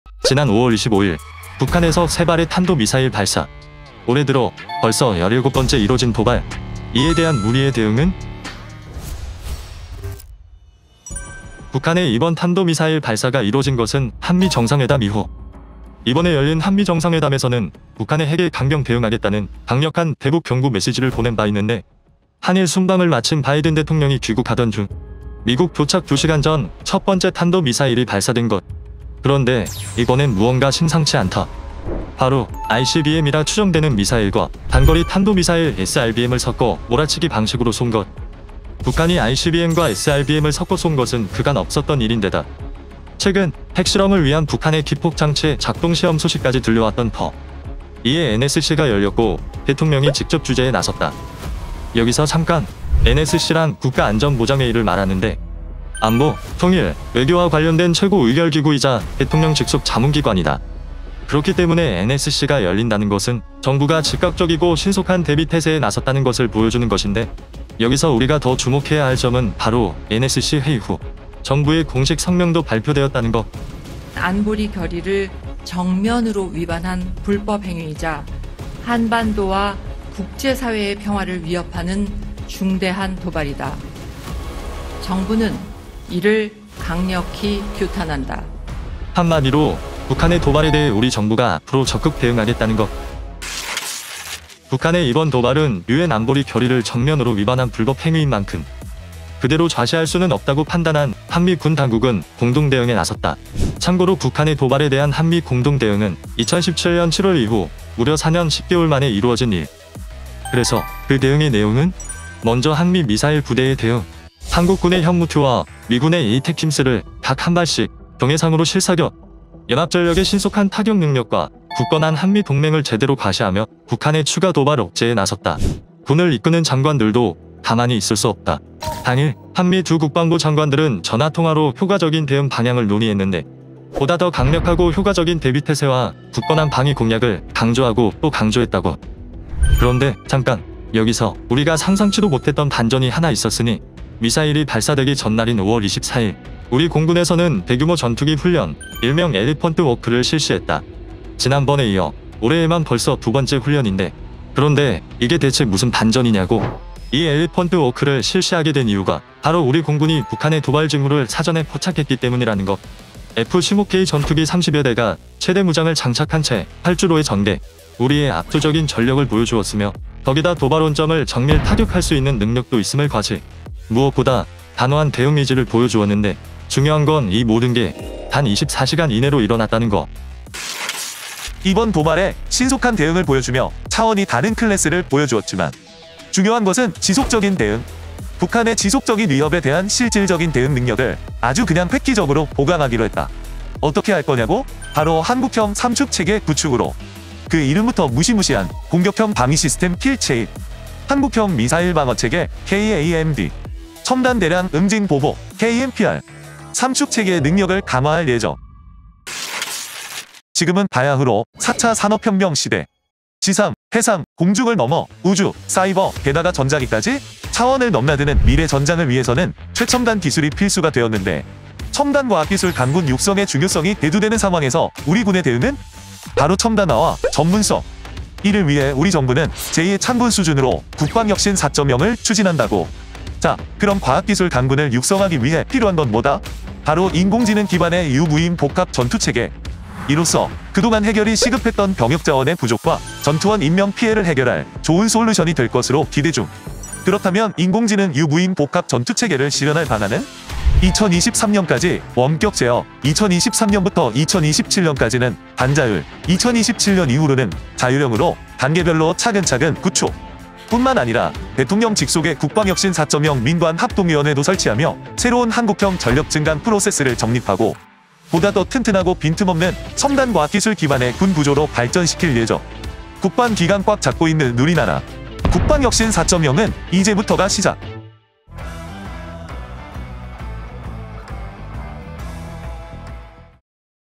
현무-Ⅱ, 에이태킴스(ATACMS) 실사격★계속되는 북한 미사일 도발에 한미 미사일 부대 타격능력 현시♣최대 무장 F-15K 엘리펀트 워크 ▶순삭특급SSTG-37
북한의 미사일 도발 이 계속되고 있습니다.
지난 5월 25일, ICBM 추정 미사일과 단거리 탄도미사일(SRBM)을
연달아 발사하며 올해만 무려 17번째 ...

한미 미사일 부대 는 북한의 계속되는 탄도미사일 도발에 대응해
한국군의 현무-Ⅱ와 미군의 에이태킴스(ATACMS)로
연합 지대지 미사일 실사격을 실시하며 이를 통해 북한의 추가 도발을
억제하기 위한 연합전력의 신속한 타격 능력을 보여줬는데요.
또 우리 공군의 F15K 전투기 30여 대가 최대 무장 을 장착한 채
엘리펀트워크(ElephantWalk) 훈련을 실시하며 상시 대비태세를
갖추고 있다는 점을 분명히 보여줬습니다.

굳건한 한미동맹 을 기반으로 한 우리 군의 대응과
새로운 국방혁신 #4.0 의 비전!
순삭특급 에서 직접 확인해 보시기 바랍니다.